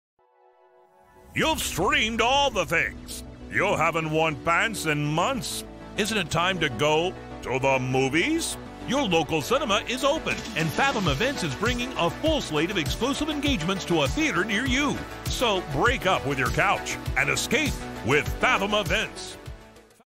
Intermission is over...
As cinemas across the country are open, Fathom wants to send a message to those who are ready to take a brief break from zoom meetings, and jogging pants, to escape to the movies. Fathom has a full slate of exclusive engagements coming to a theater near you and we thought we would try and put a smile on some faces while we let them know.